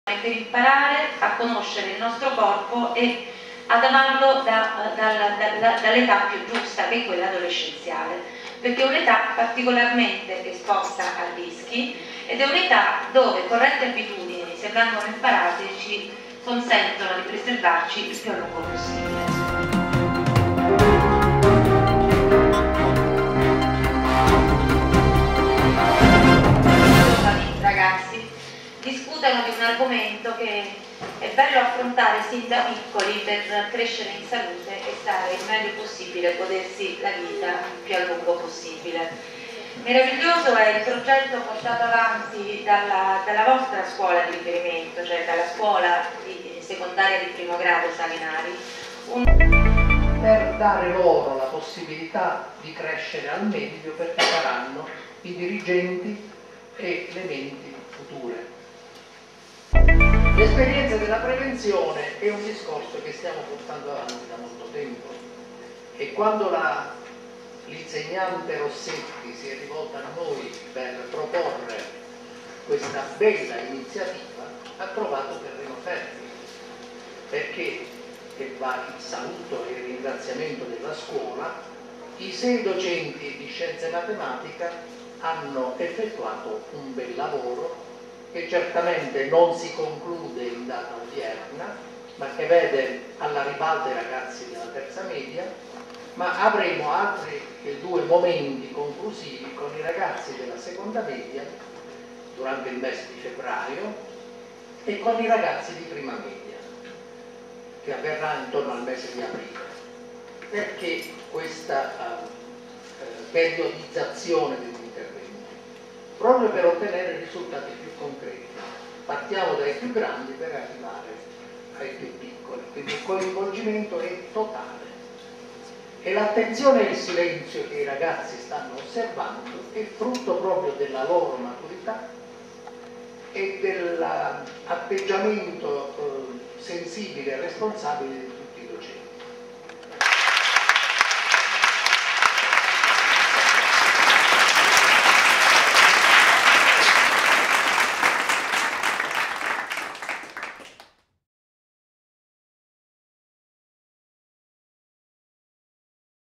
Conoscerlo per amarlo....
Un Convegno, un incontro per formare ed informare i ragazzi sulla necessità di Vivere meglio le esperienze verso la Vita Adulta.